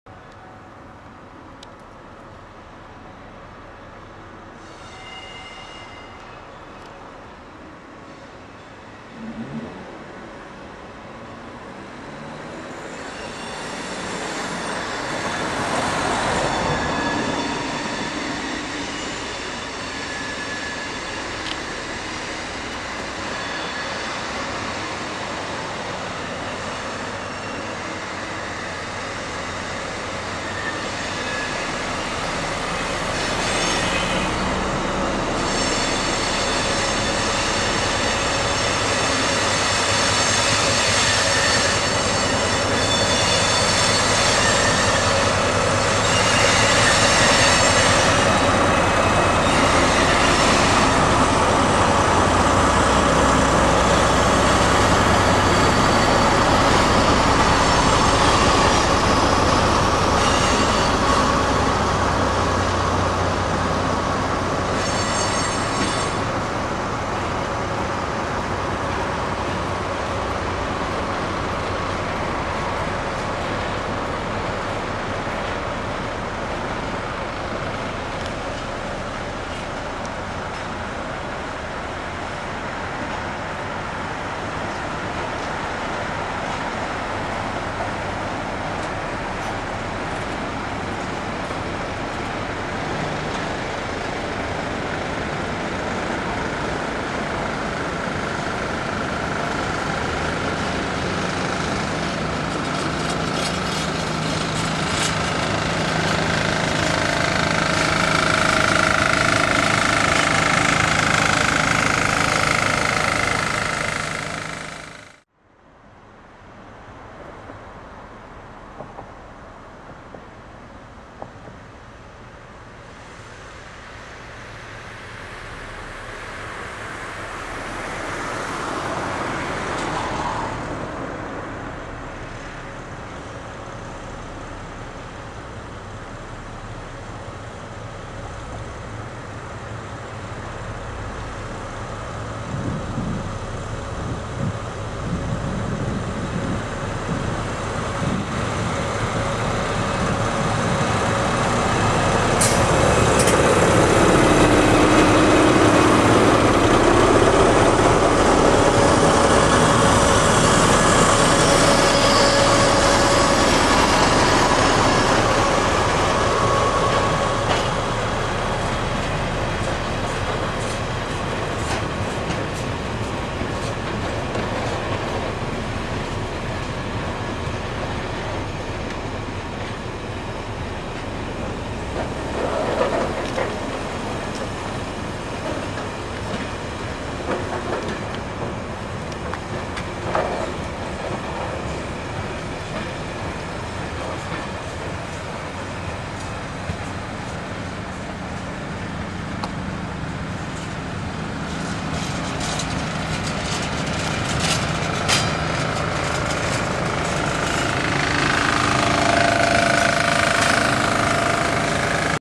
Convoi exceptionnel ferroviaire avec Stator
Départ de la gare de Belfort d'un convoi exceptionnel ferroviaire
Il s'agit d'un Stator ( un stator = alternateur nucléaire )
Il partait en direction de Chalindrey
Se sont 2 locomotives de type BB 69000 Fret ( BB 69425 et BB69432 ) qui tire se lourd convoi de l'entreprise STSI